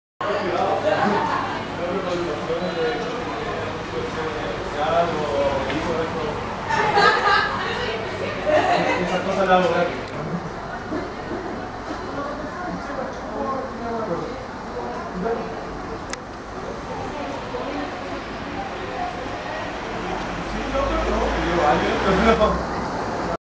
Maniquí
Intervención artística urbana realizada por Nepeta la weta.

Concepción
Febrero, 2019.